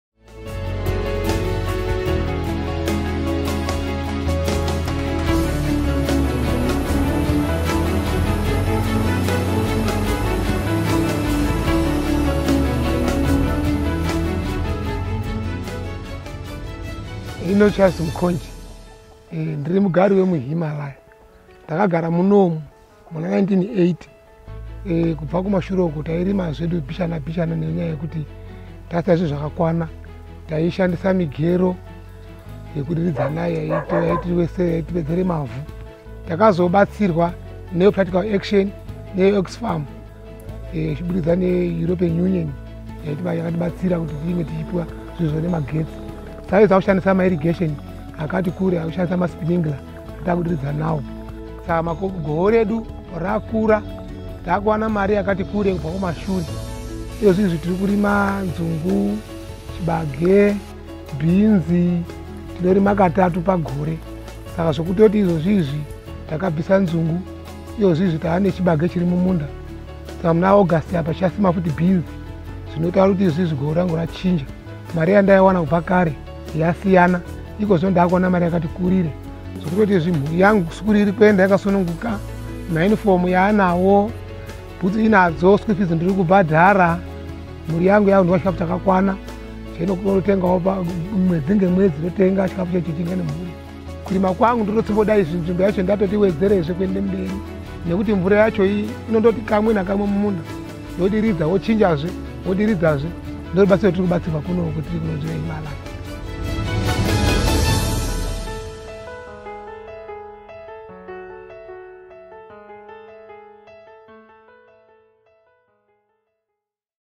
Sustainable energy for improved food security | RuSED
Charles Mukonji discusses the impact that the Rural Sustainable Energy Development Project (RuSED) has had in his area. This involves farming methods, a micro-hydro system for generating electricity and improved sprinkler irrigation.

Country: Zimbabwe
Language:  Shona